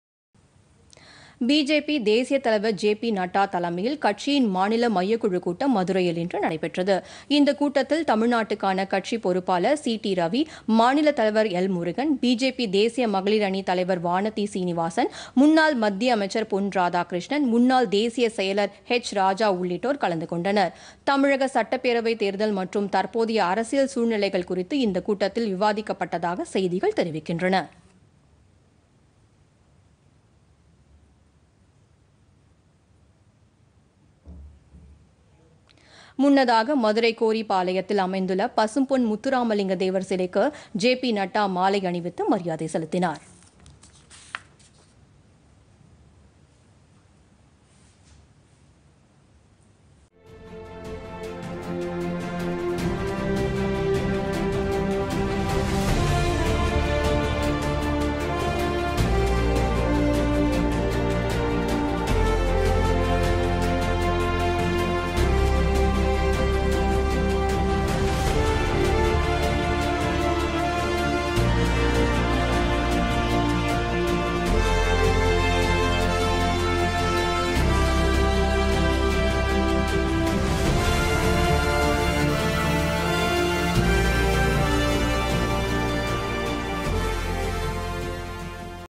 பிஜேபி மாநில மையக்குழு கூட்டம்PodhigaiTamilNews
மதுரையில் பிஜேபி தேசியத் தலைவர்  ஜே பி நட்டா தலைமையில் கட்சியின் மாநில மையக்குழு கூட்டம் - சட்டப்பேரவைத் தேர்தல், அரசியல் நிலைமைகள் குறித்து ஆலோசனை.